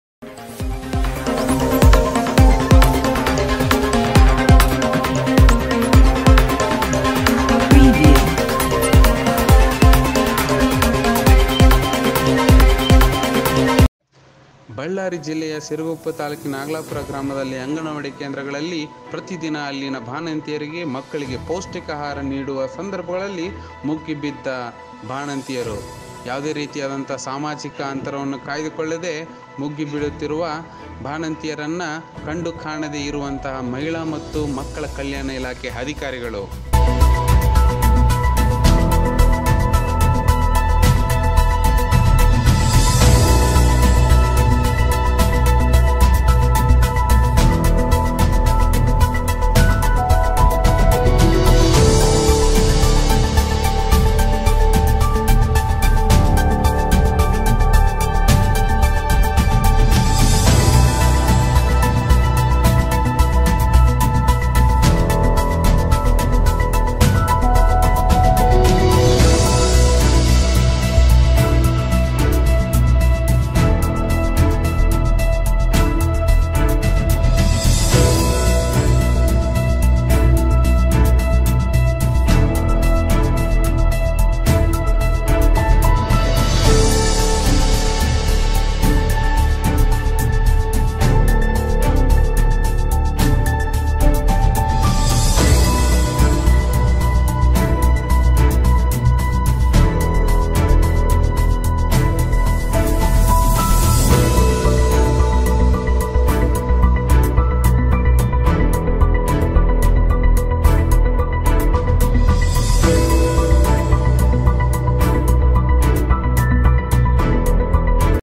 ಆಹಾರ ಪದಾರ್ಥಗಳಿಗಾಗಿ ಮುಗಿಬಿದ್ಧ ಬಾಣಂತಿಯರು ಅಧಿಕಾರಿಗಳ ನಿರ್ಲಕ್ಷ್ಯ.
ಸಿರುಗುಪ್ಪ ತಾ ನಾಗಲಾಪುರ ಗ್ರಾಮದಲ್ಲಿ ಅಂಗನವಾಡಿ ಕೇಂದ್ರಗಳಲ್ಲಿ  ಪ್ರತಿದಿನ ಅಲ್ಲಿನ ಬಾಣಂತಿಯರಿಗೆ,ಮಕ್ಕಳಿಗೆ ಪೌಷಿಕ ಅಹಾರ ನೀಡುವ ಸಂದರ್ಭದಲ್ಲಿ ಬಾರಿ ಜನಸಂಖ್ಯೆ ಸೇರಿರುವುದು  ಆಹಾರ ವಿತರಣೆ ಮಾಡುವ ಸಂದರ್ಭದಲ್ಲಿ ತಾಲೂಕಿನ ಮಹಿಳಾ ಮತ್ತು ಮಕ್ಕಳ ಕಲ್ಯಾಣ ಇಲಾಖೆ ಅಧಿಕಾರಿ ಸಮ್ಮುಖದಲ್ಲಿ  ಆಹಾರ ಪದಾರ್ಥಗಳು ವಿತರಣೆ ಮಾಡುತ್ತಿರುವುದು ಬಾರಿ ಜನಸಂಖ್ಯೆ ಸೇರಬಾರದು ಎಂದು ಹೇಳಿ ಅಧಿಕಾರಿಗಳು ಹೇಳುತ್ತಾರೆ ಅದರೆ ಅಧಿಕಾರಿಗಳು ಲಾಕ್ ಡೌನ್ ಉಲ್ಲಂಘನೆ ಮಾಡಿರುವುದು. ಕಂಡುಬಂದಿದುವುದು.